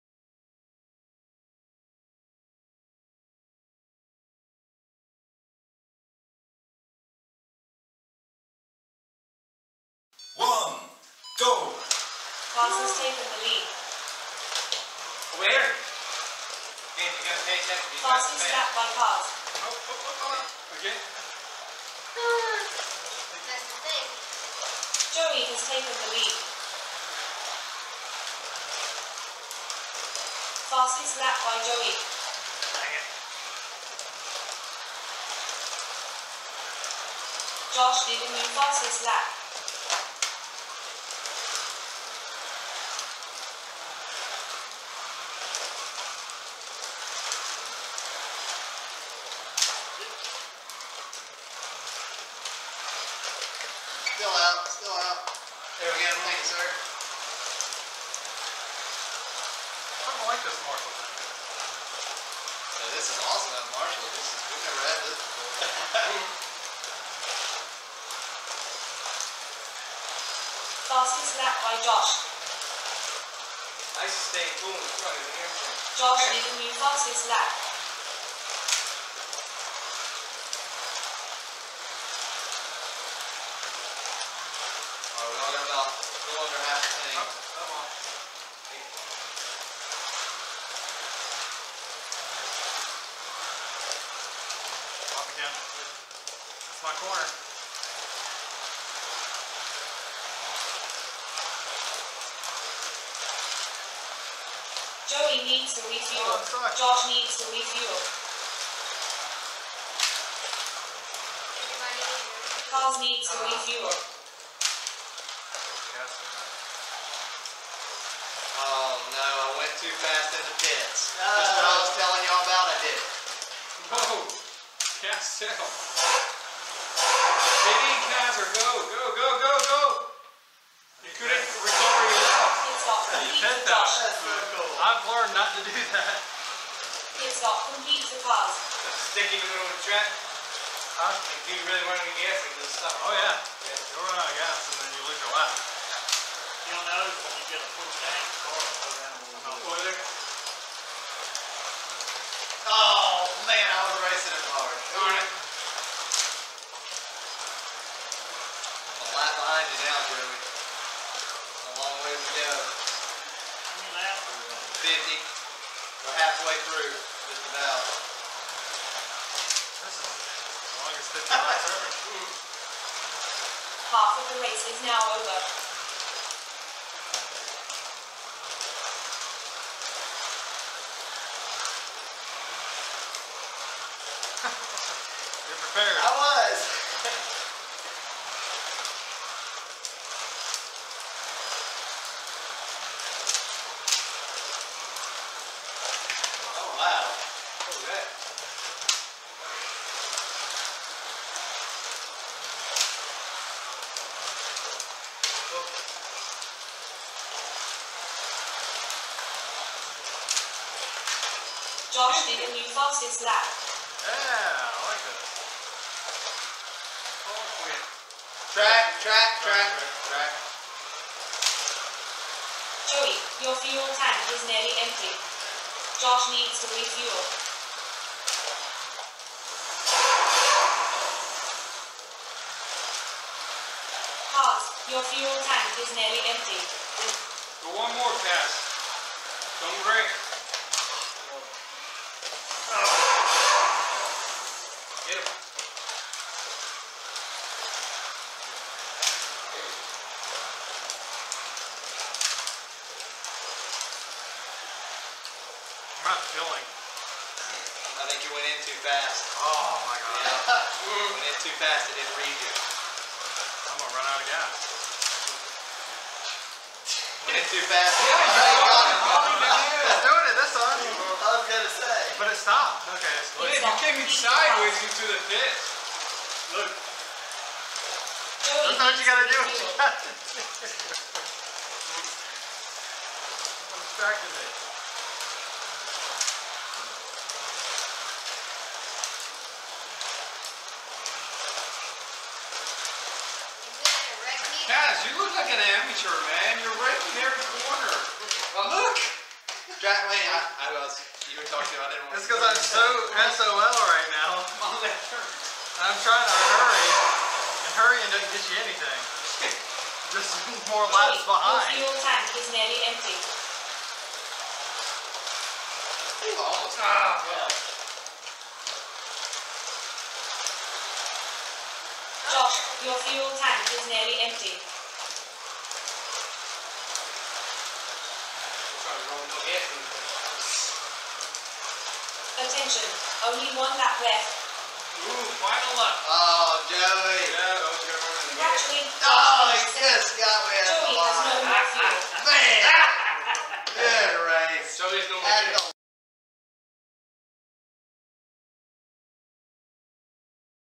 Carrera Digital 1/32 DTM 50 Lap Race (4-28-17)
Our first race with marshals! Thanks goes to James, Matthew, Nathan, and Rafael for marshaling this race.